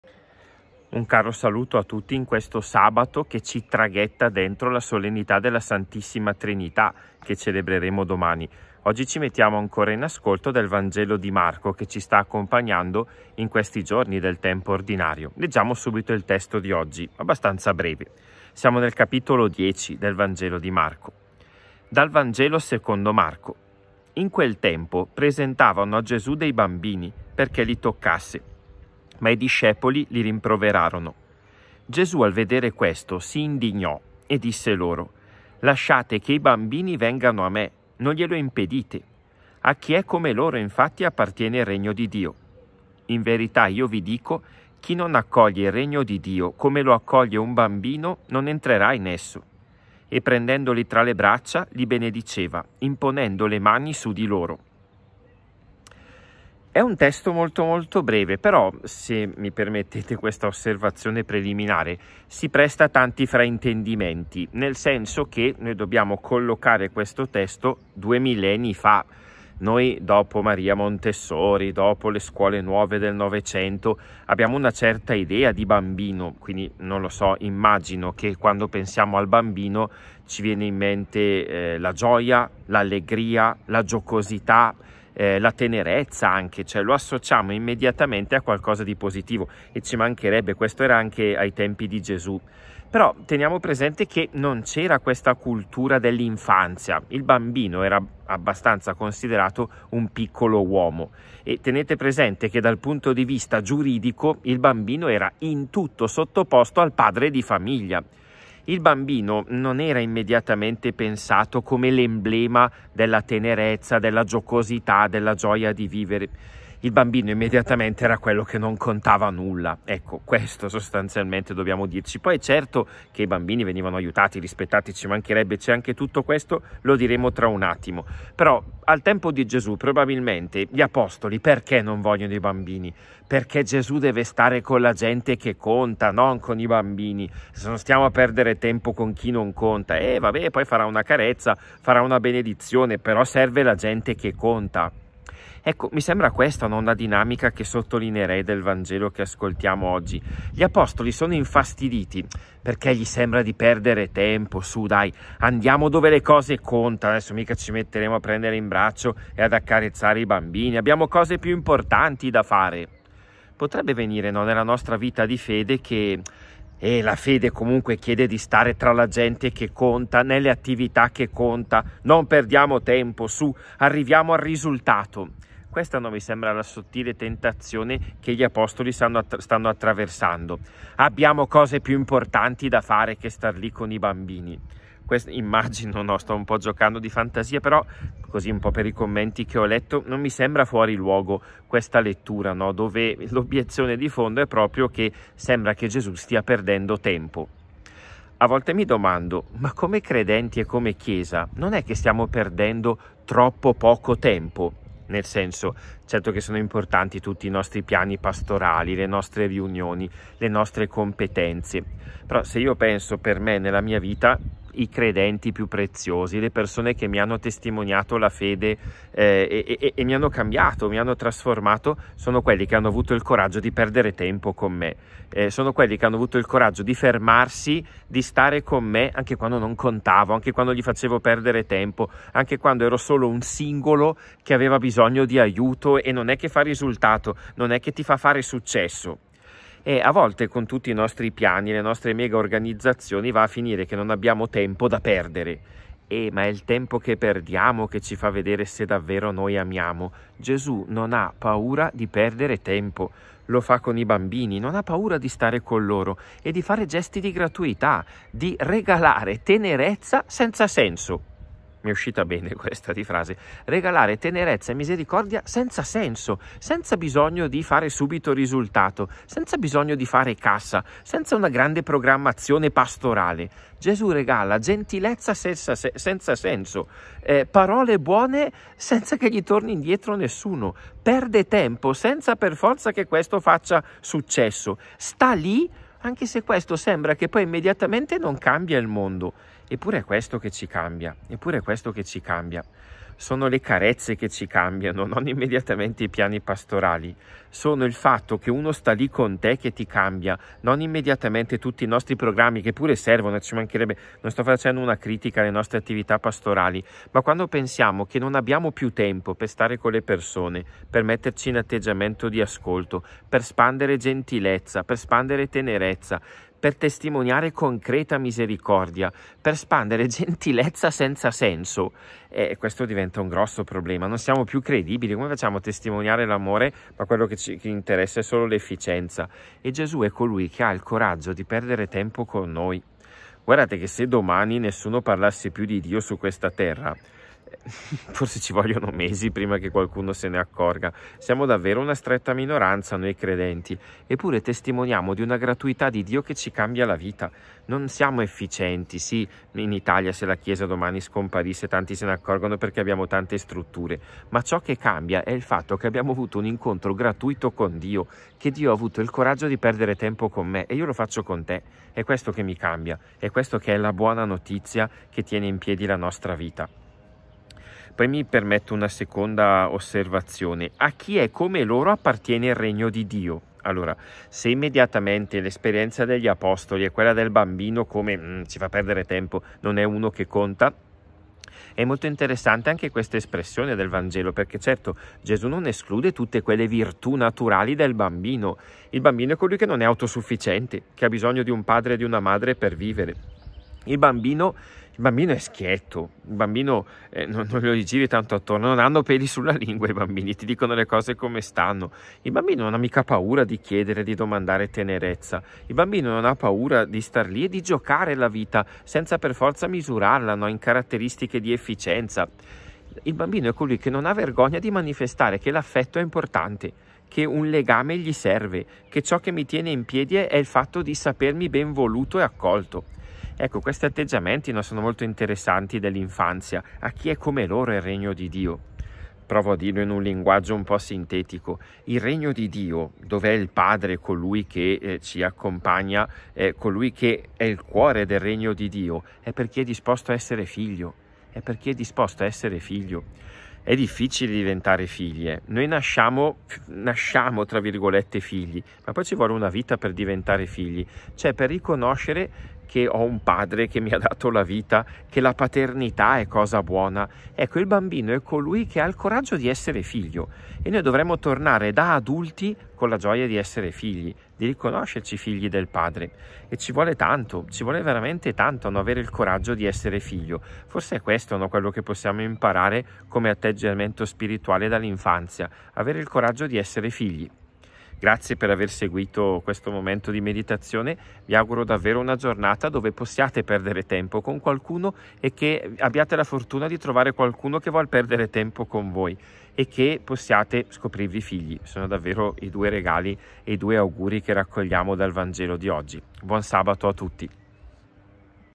Il VANGELO di sabato 25 maggio
Lettura e meditazione del Vangelo che la liturgia ci regala per sabato 25 maggio.